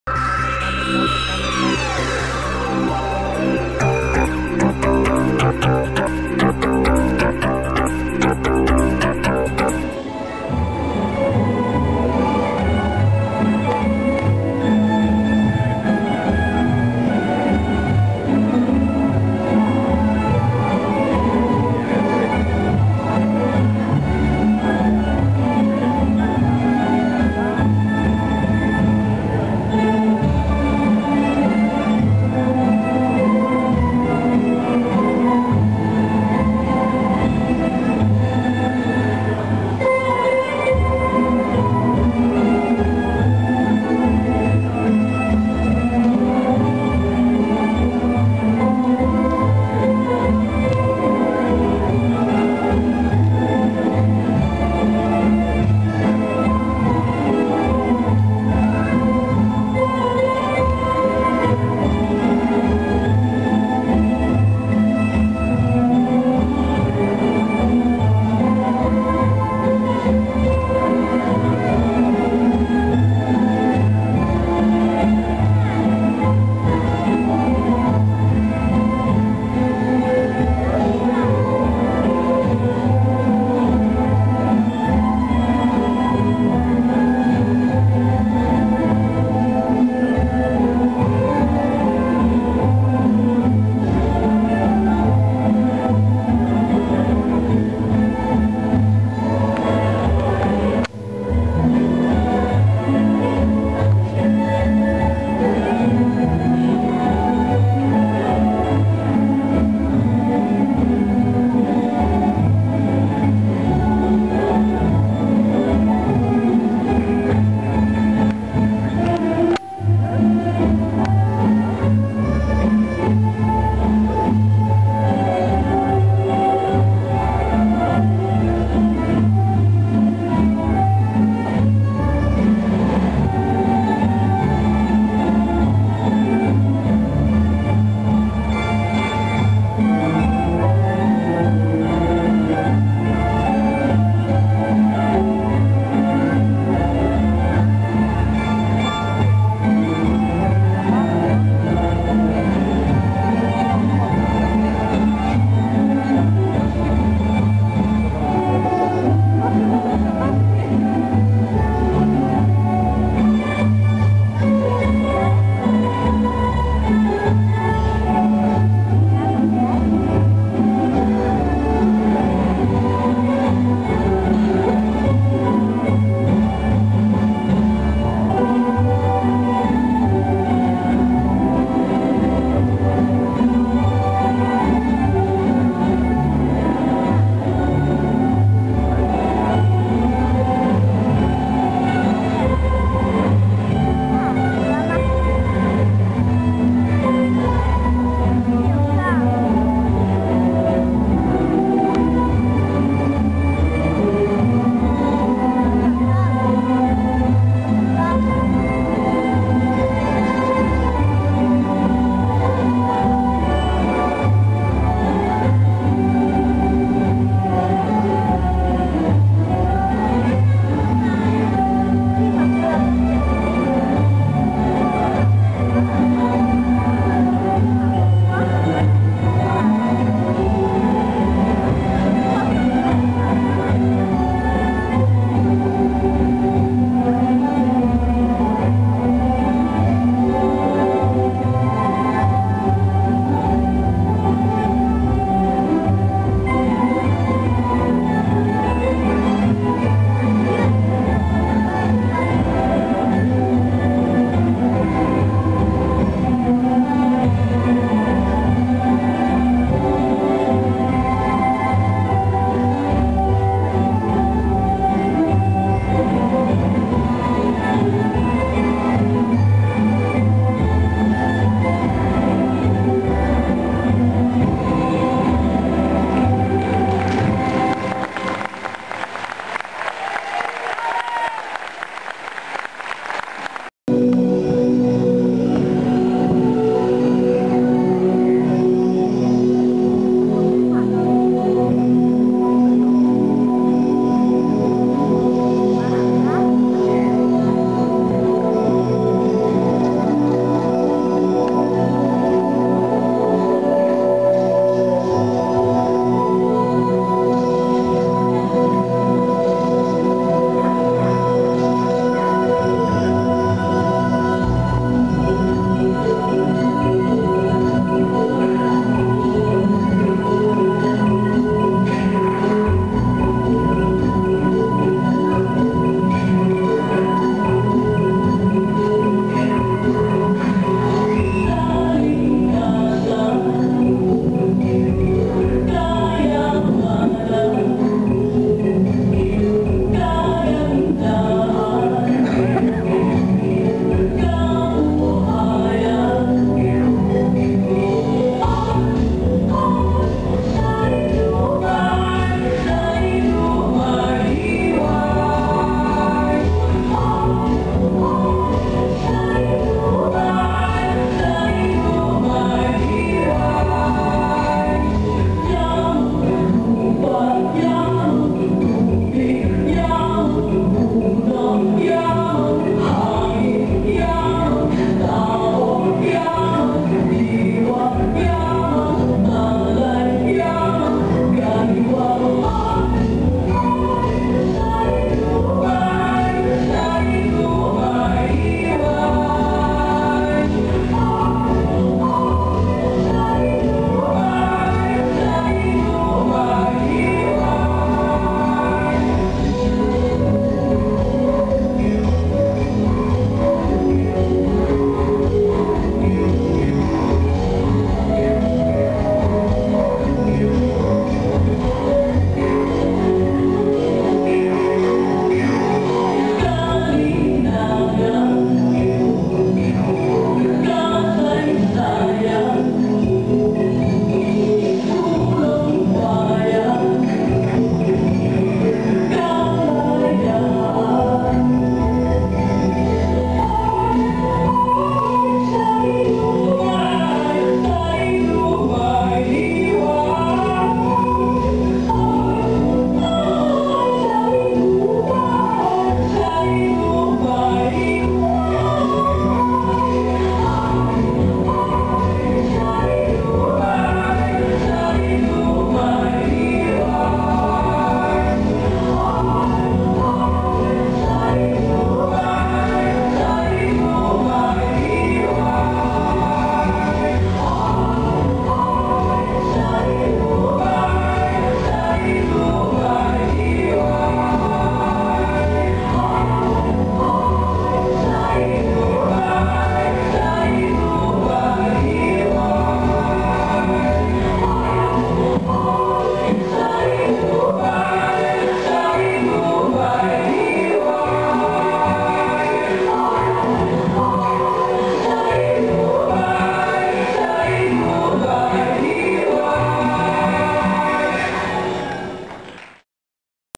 FILIPPINE - danza delle candele
L'esibizione del gruppo delle Filippine alla 5^ edizione del FESTIVAL delle CULTURE all'Auditorium Zanon di Udine. Il festival è il tradizionale appuntamento di arte, musica, letteratura, danza organizzato da UCAI fvg, l Unione delle Comunità ed Associazioni di Immigrati, un coordinamento di 46 diverse organizzazioni con sede a Udine.